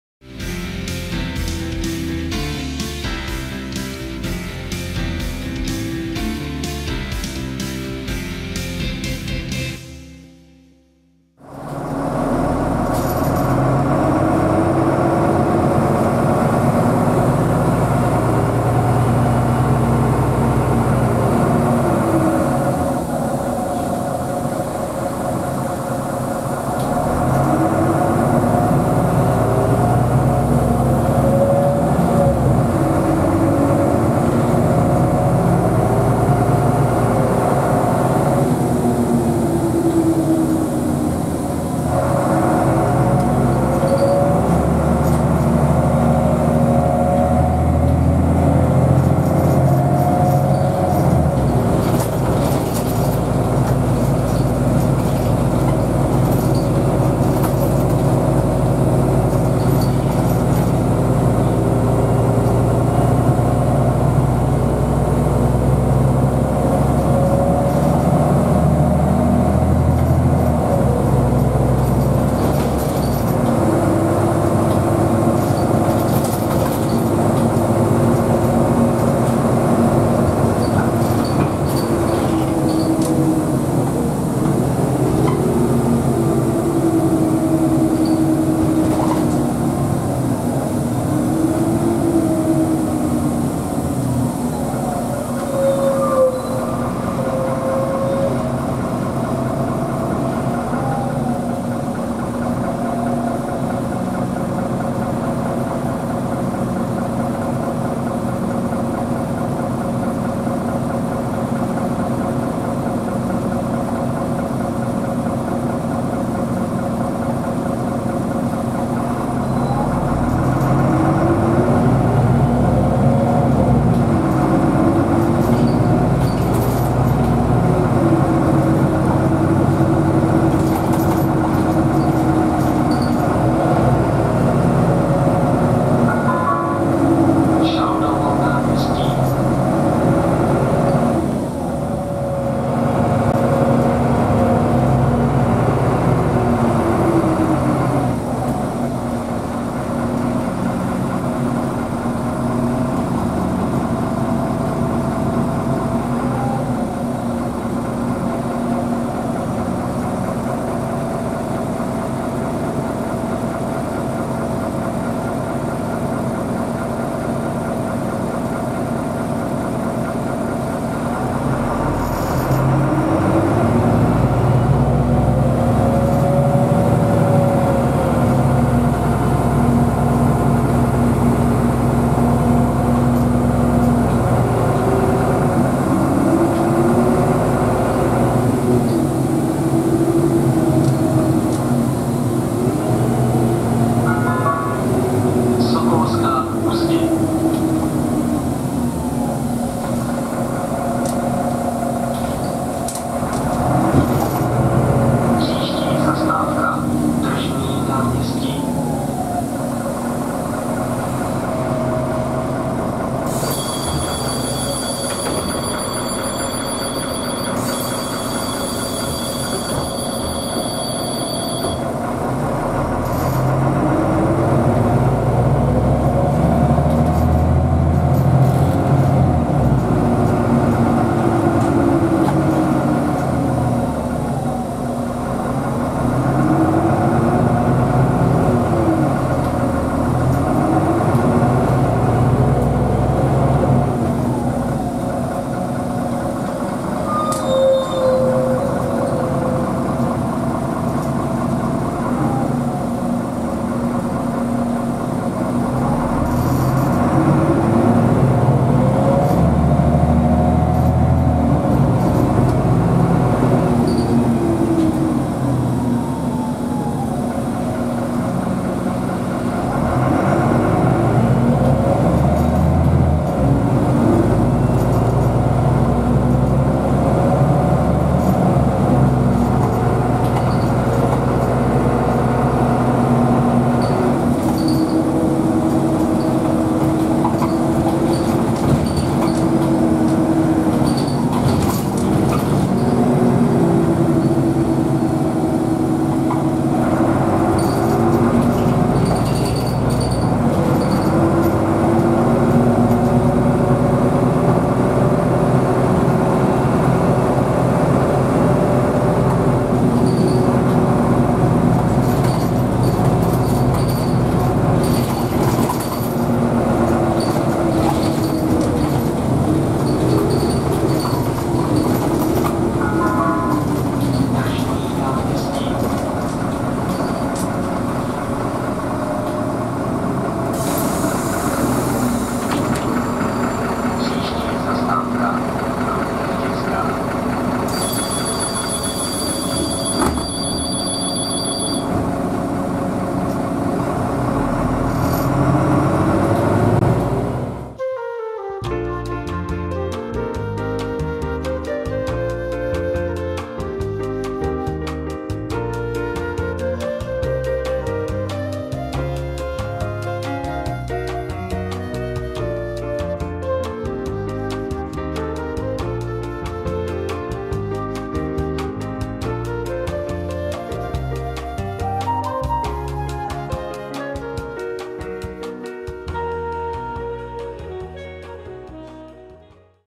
Irisbus Citybus 18M 2081 (ex.Zlín) | 14: Fügnerova - Tržní náměstí | DPMLJ | Liberec
In this video you can see a ride on board the bus Irisbus Citybus 18M 2081 on line 14 in Liberec in the Czech Republic. This car, manufactured in 2002, was put into normal operation in Liberec in July 2019. In May 2021 was the bus shut down and subsequently took out of service in October of the same year.

V tomto videu je zachycena jízda autobusem Irisbus Citybus 18M 2081 ev.č.307 (ex. DSZO 820) na lince 14 v Liberci. K vidění je úsek v trase Fügnerova - Šaldovo náměstí - Sokolská u zdi - Tržní náměstí.
Tento vůz, rok výroby 2002, byl v Liberci zařazen do běžného provozu v červenci 2019. V květnu 2021 byl autobus odstaven a následně v říjnu téhož roku vyřazen.
Na lince 14 se tehdy jednalo o jediný spoj, na který byl nasazen kloubový autobus. Spoj byl veden jako linka 14 do Pavlovic na Letnou, kde byl přečíslován na linku 12 a pokračoval směr Zelené Údolí přes Polní. Z důvodu nedostatku kloubových autobusů, respektive jejich jiného nezbytného využití, jsou dnes na některé takovéto spoje, kdy mění autobus linku či trasu, vysílány pouze krátké dvanáctimetrové vozy.
O zajištění veřejné hromadné dopravy na území města Liberce se stará DPMLJ (Dopravní podnik měst Liberce a Jablonce nad Nisou, a.s.).

In diesem Video kann man eine Mitfahrt im Irisbus Citybus 18M 2081-Bus auf der Linie 14 in Liberec in Tschechien ansehen.